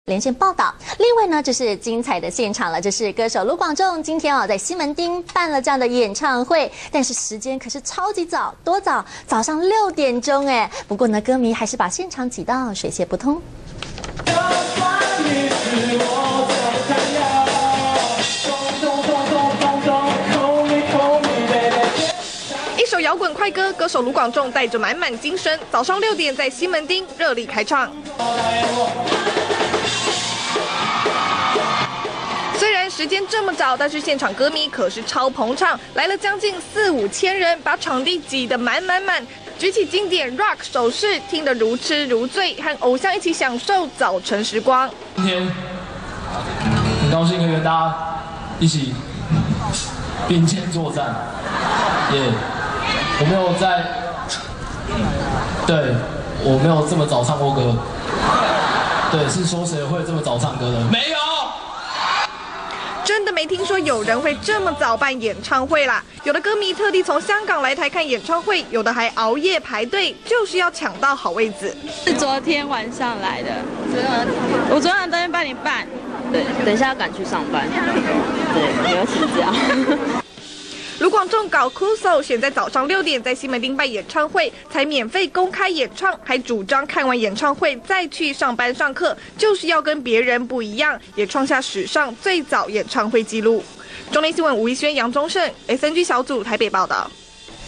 清晨6點開演唱會！聽完盧廣仲 趕上班上課
中天新聞》9日清晨6點，乘著超大貨櫃車進場，盧廣仲照慣例穿著短褲白襪、背上小吉他，在西門紅樓前廣場開唱，現場歌迷人山人海，時間一到立刻衝出黃色封鎖線，奔向台前。